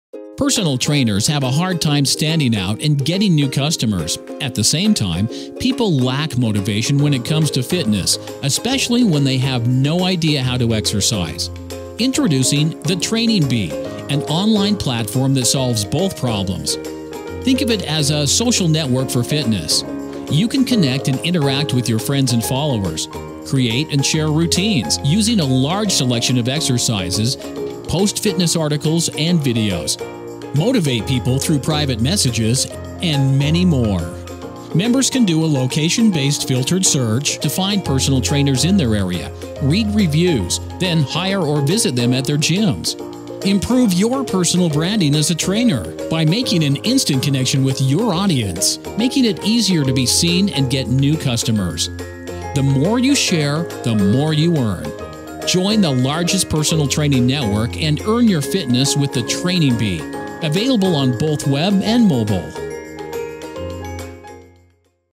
The Training Bee
A fitness web + app where personal trainers share their fitness routines, interact with followers and are rated, creating an innovative fitness community.